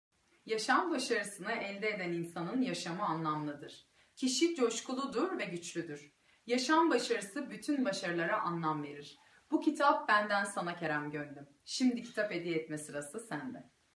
Elif Gönlüm
Elif Gönlüm Herkese Kitap Vakfı'nın başlattığı Kitap Hediye Günü projesine Kerem Gönlüm'e kitap hediye ederek katıldı.